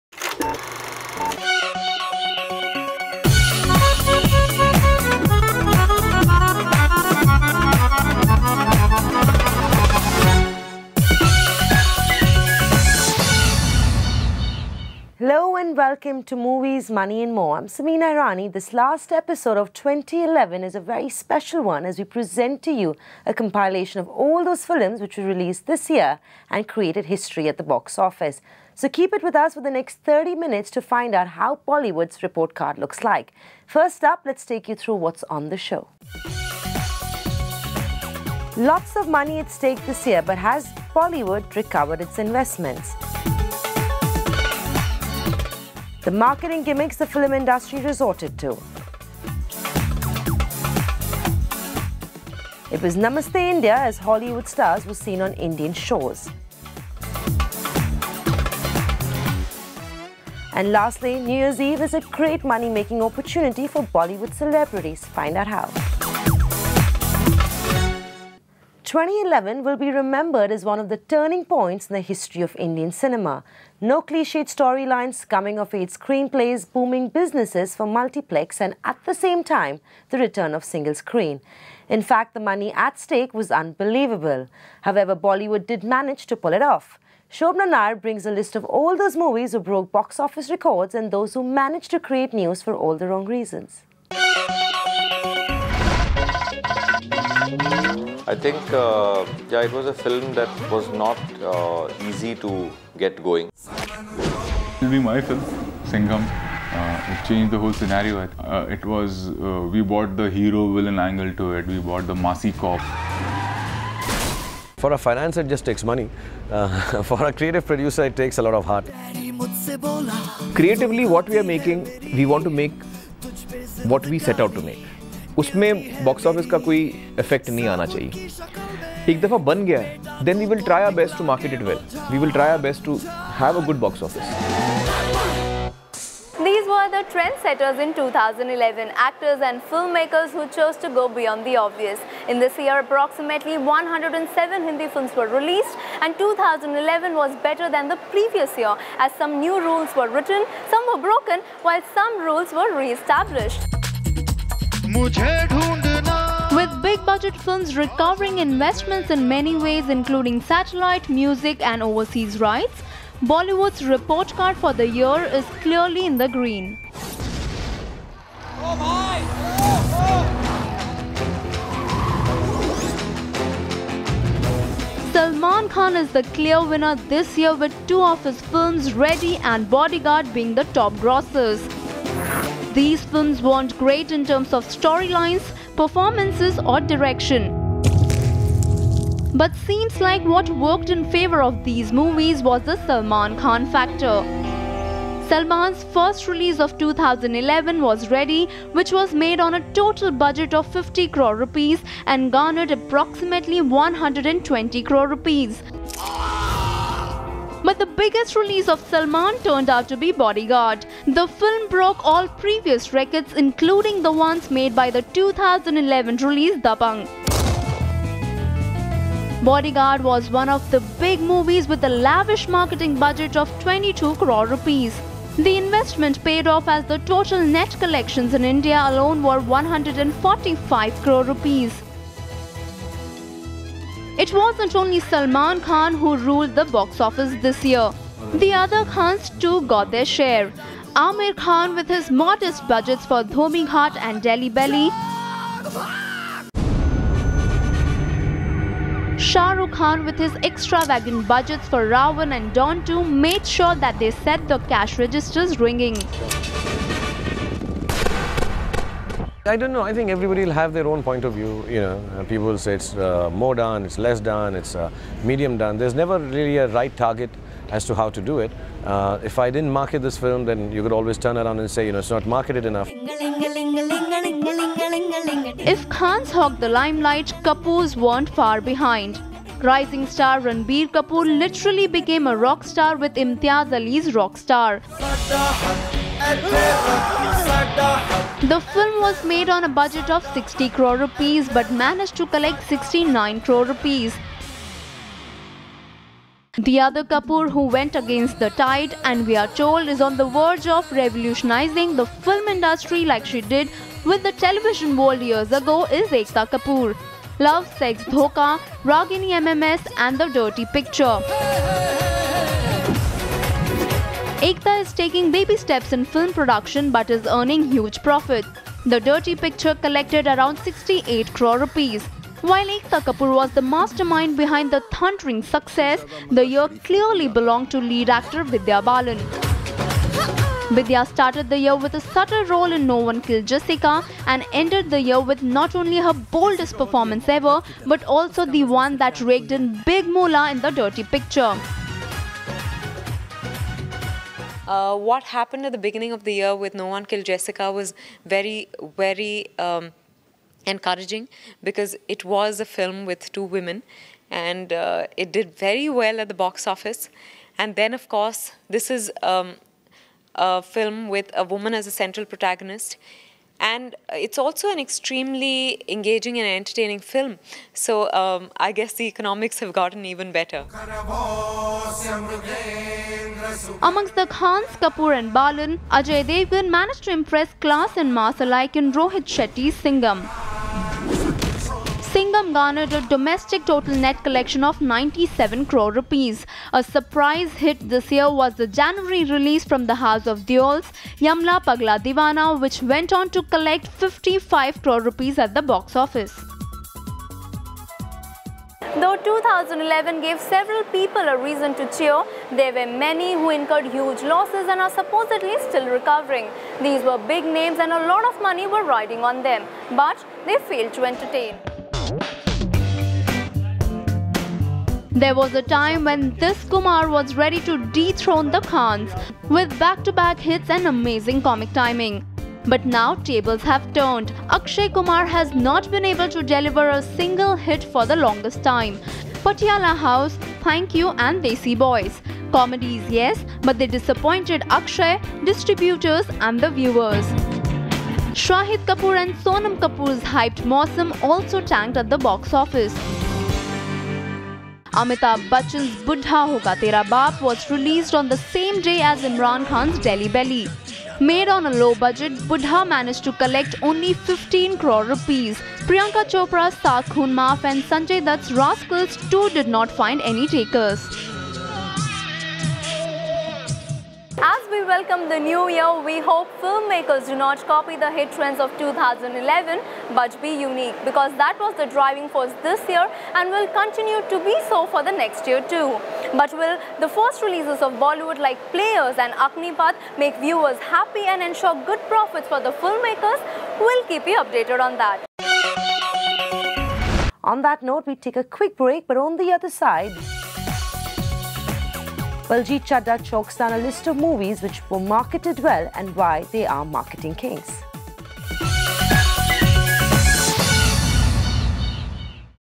Movies Money & More: Hits Of 2011
List of movies who broke box office records and those who manage to create news.